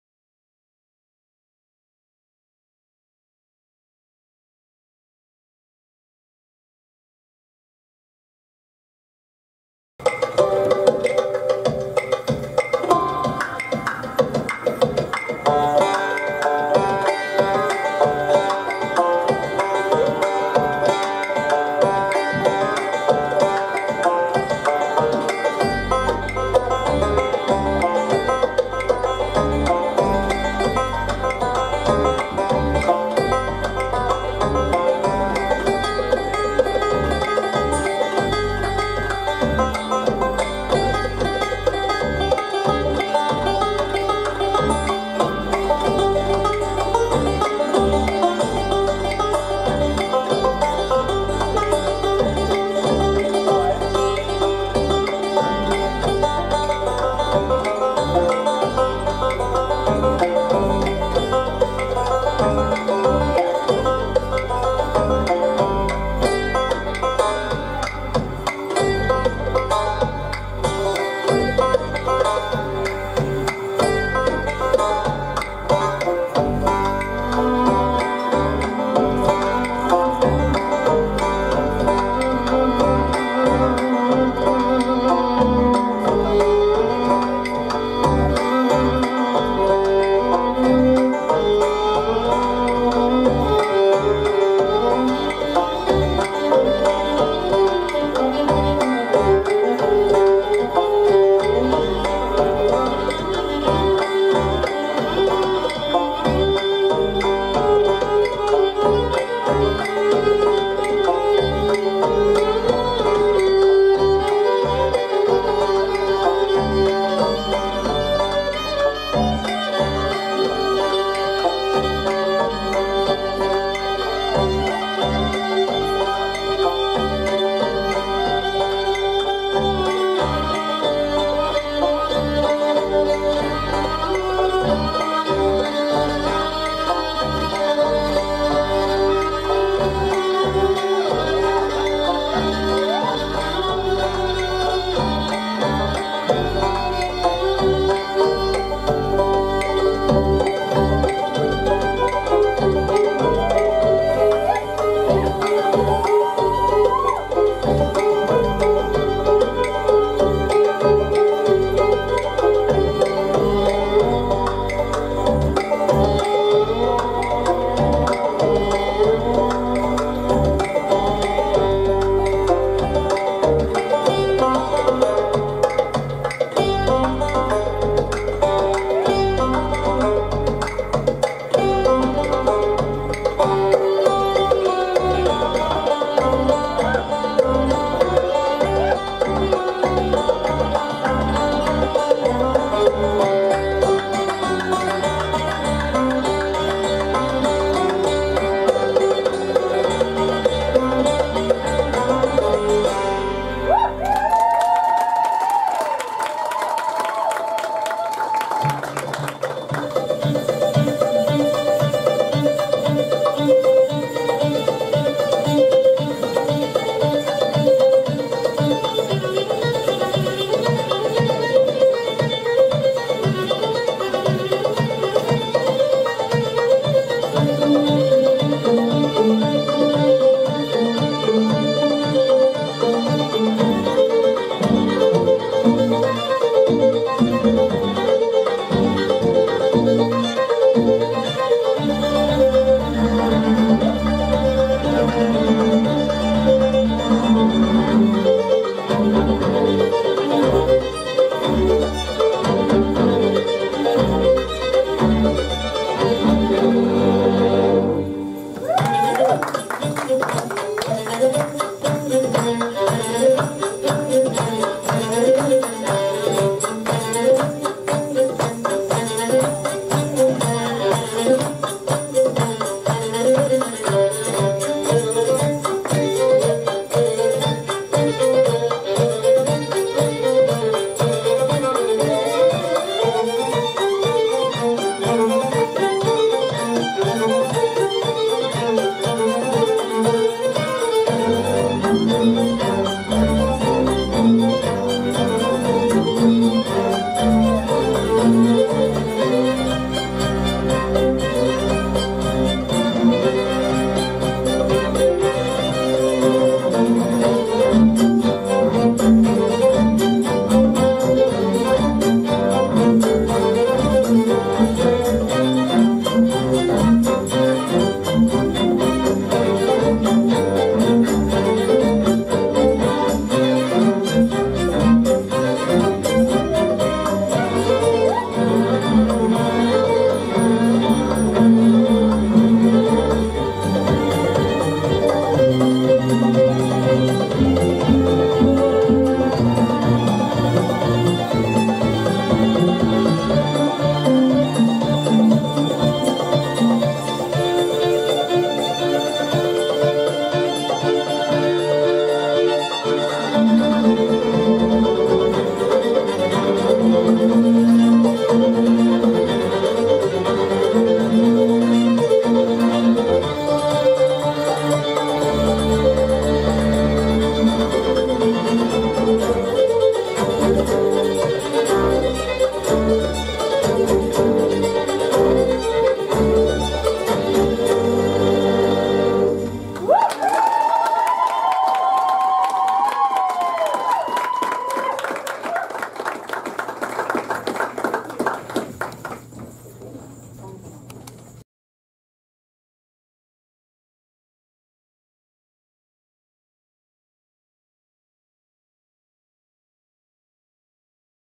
Santa Cruz Raks Oct 2015 - Chelsie
This was my first time dancing in the Santa Cruz Raks Community Showcase hosted by *Setareh* It was so much fun and I enjoyed every second of it!

My dancing was entirely improvised.